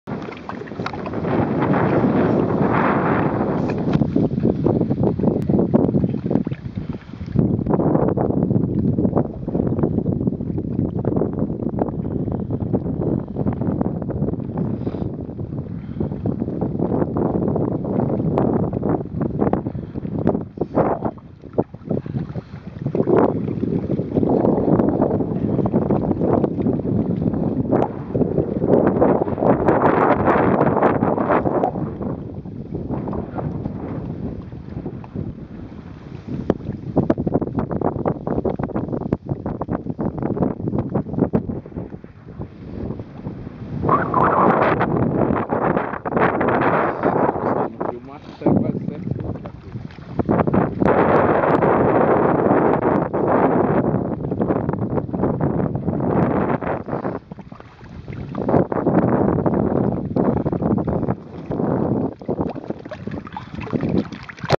O que restou da casa dos meus avós
A casa centenária dos meus avós onde meus, lar dos meus antepassados, depois de ser atropelada pelo progresso e afogada por uma barragem. Debaixo d'água no sertão de Itapipoca as lembranças da minha infância, das minhas férias escolares que eu passava no sítio. Sertão de Itapipoca, Ceará, 2012.